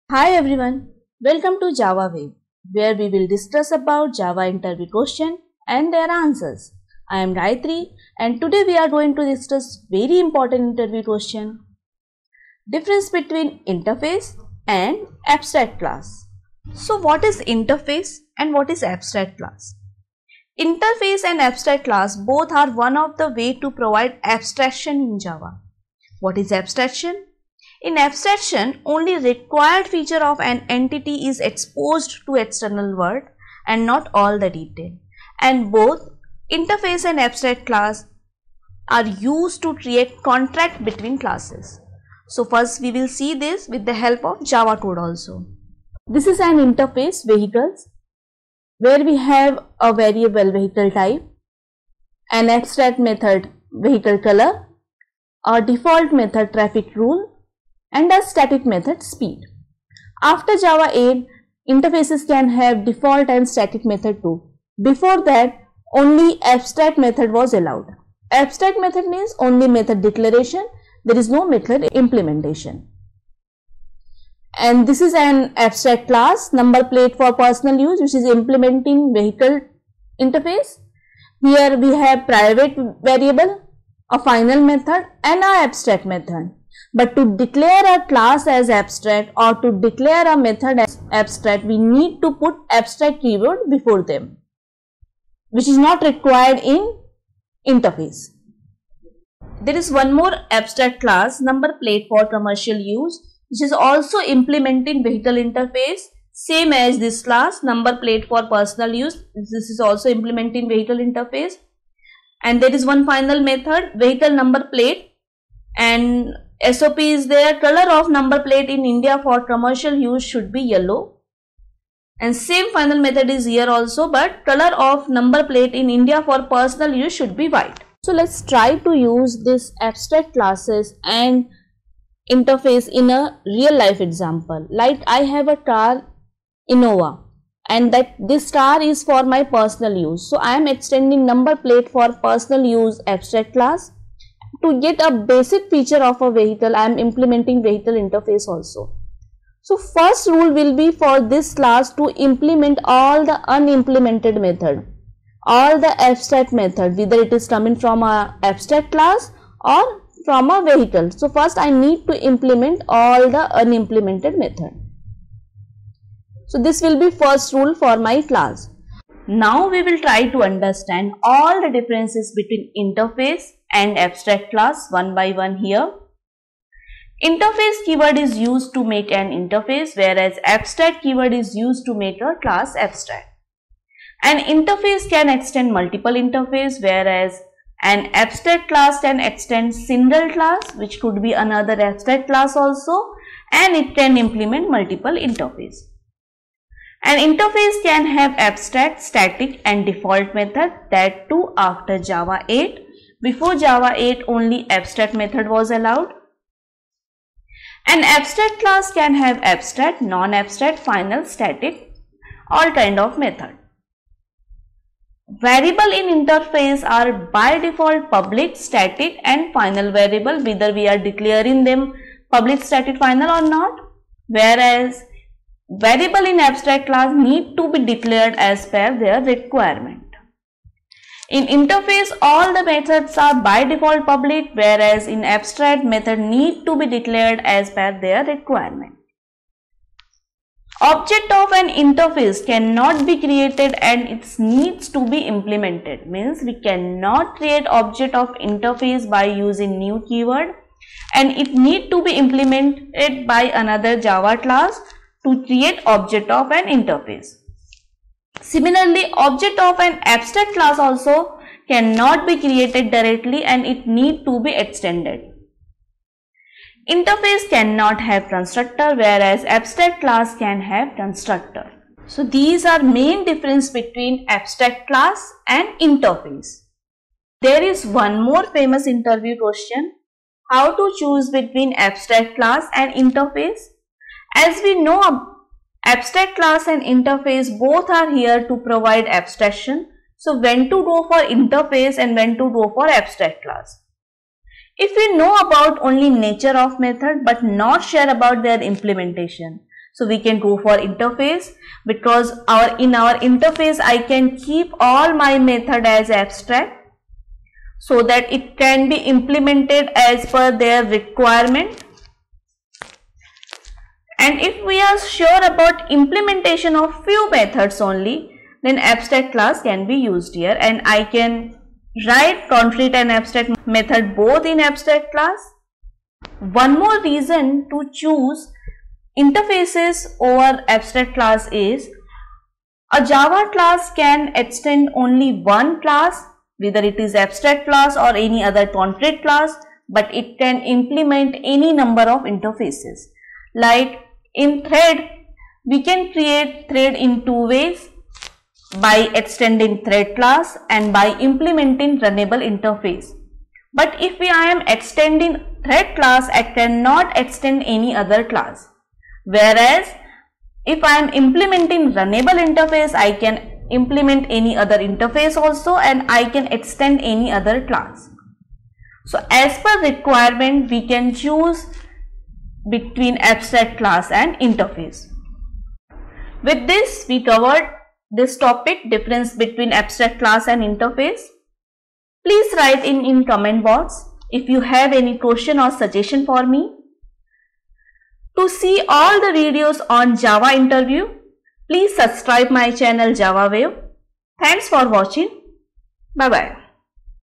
Difference Between Interface And Abstract Class | Core Java Interview Question
Difference Between Interface And Abstract Class is explained in this video. Difference between interface and abstract class in java   8. When to use abstract class and when to use interface in Java.

Java tutorial

Video for difference between Method overloading and Method Overriding
   Or

Video for Differenace between String, StringBuffer and StringBuilder in java

Introduction to collection framework